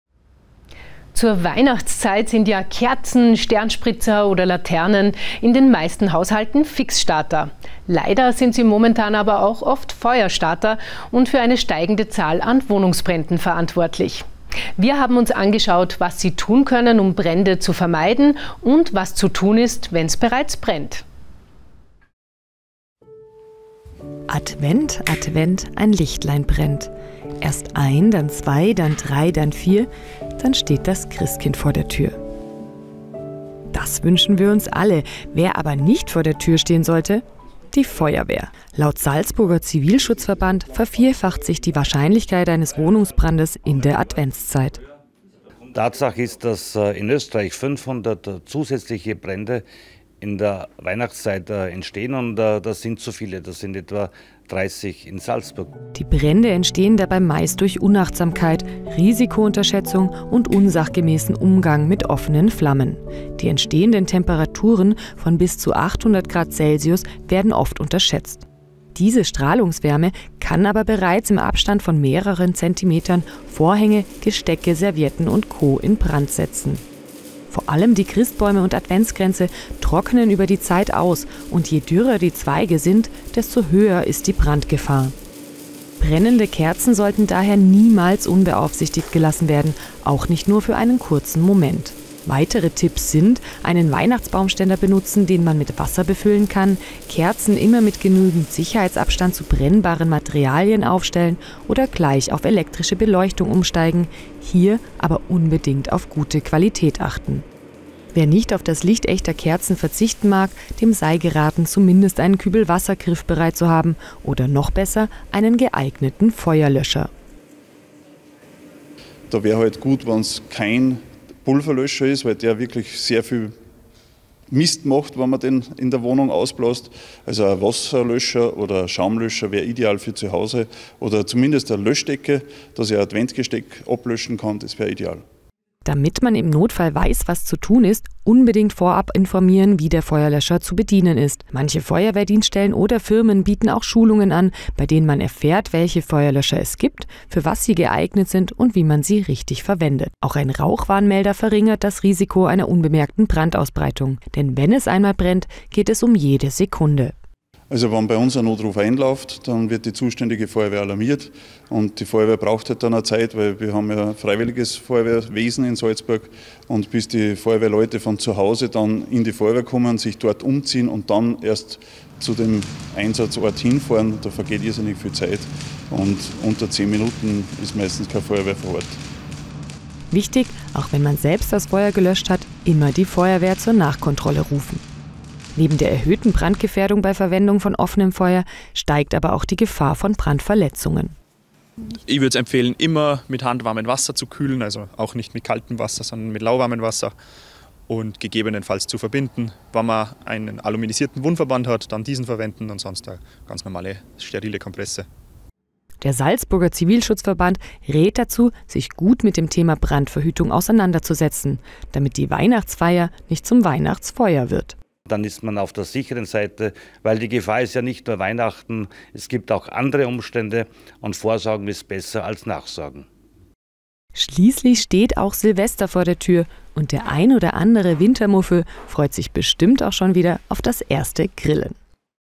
Advent, Advent, der Christbaum brennt!
Zur Weihnachtszeit sind ja Kerzen, Sternspritzer oder Laternen in den meisten Haushalten Fixstarter.
Leider geht damit aber auch eine steigende Zahl an Wohnungsbränden einher. Wir haben uns angeschaut, was Sie tun können, um Brände zu vermeiden und was zu tun ist, wenn\'s bereits brennt.